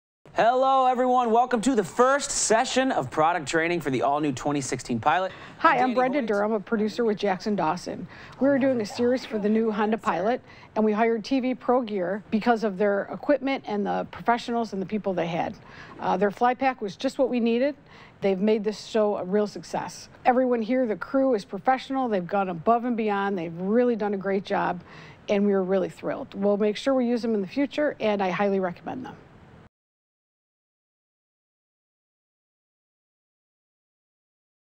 Brenda Durham - Jackson Dawson Testimonial
Brenda Durham (Jackson Dawson) hired TV Pro Gear to shoot their live streaming video production for Honda training.